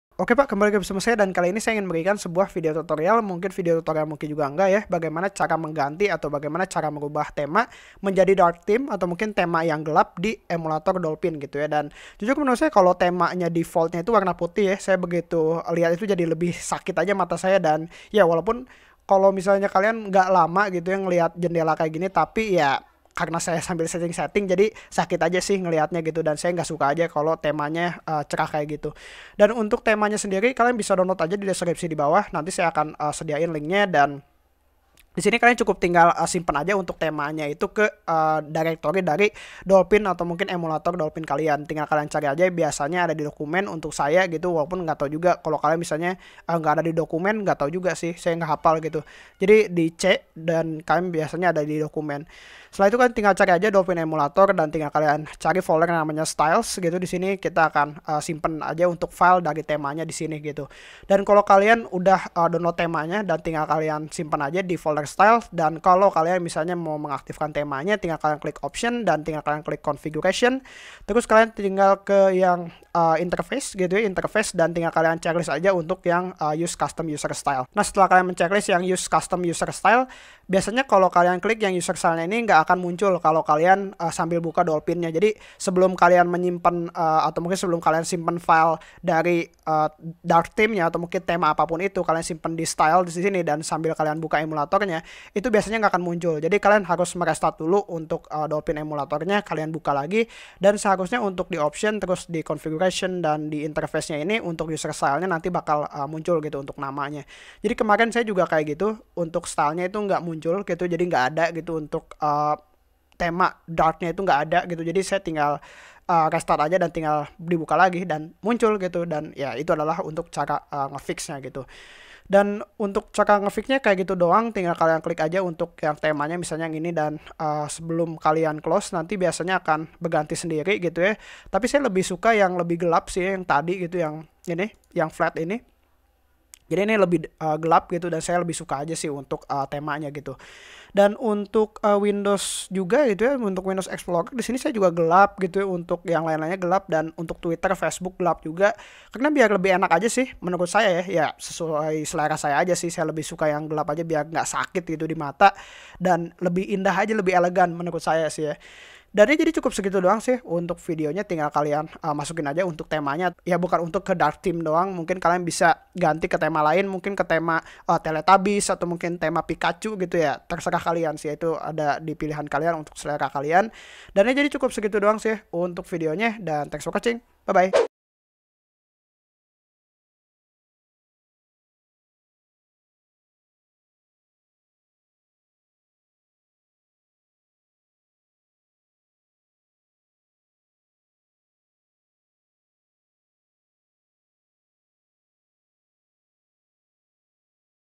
Cara Ganti Tema di Dolphin Emulator!(Jadi Dark Theme)
oke pak,kali ini saya akan memberikan sebuah video tutorial bagaimana cara mengganti tema di emulator dolphin,saya mengganti tema doplhin saya ke dark/night.